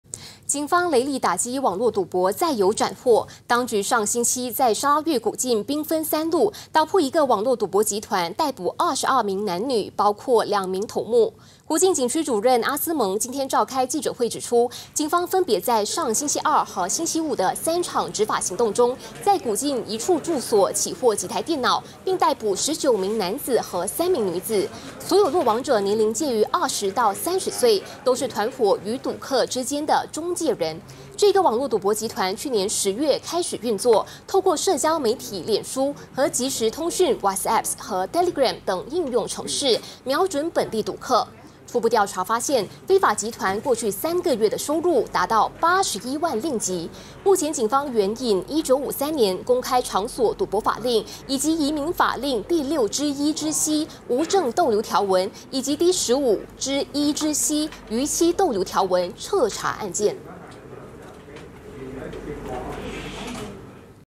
【砂警捣破网络赌博集团 社网揽客3个月赚81万】 [20220110]
警方雷厉打击网络赌博再有斩获。当局上星期在砂拉越古晋兵分三路，捣破一个网络赌博集团，逮捕22名男女，包括两名头目。